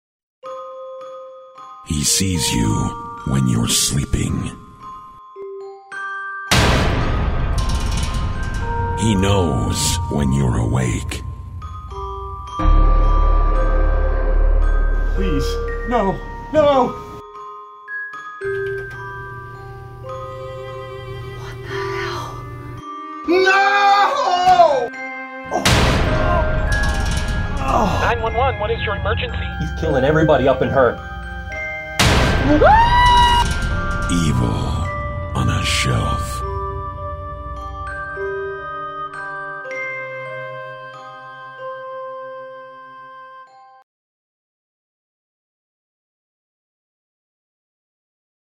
Evil Elf on the Shelf
We've never been a fan of "Elf on a Shelf." It's a little too creepy for us. Now we know why.